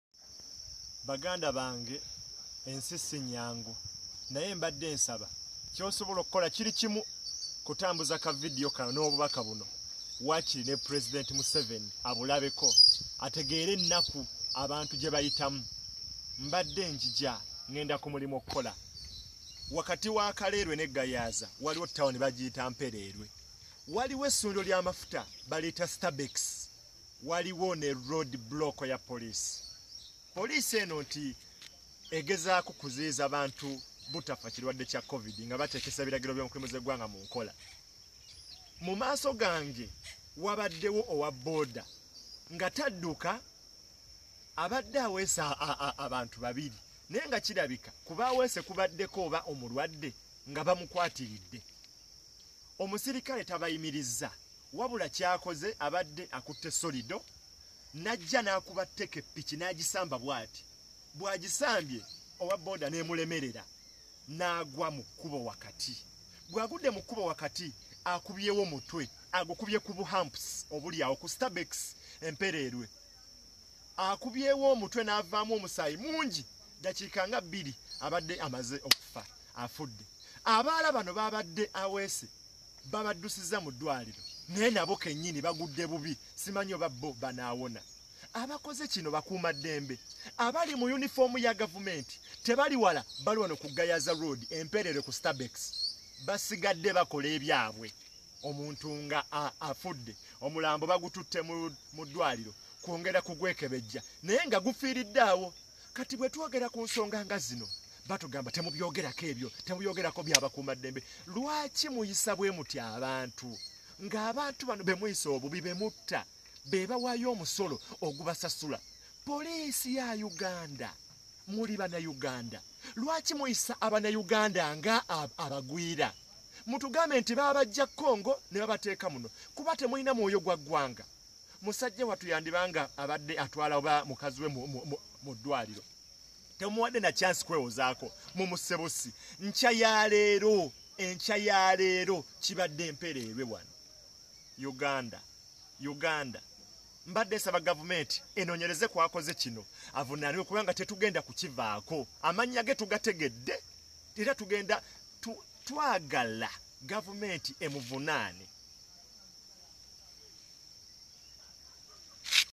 Police ye mpereerwe esse owabooda abadde atwaala omulwaade kubdwaliro lya KCCA e komamboga!
Police ye mpereerwe esse owabooda abadde atwaala omulwaade kubdwaliro lya KCCA e komamboga! Bamukubye teke nagwa Omutwe negwatikira ku Hump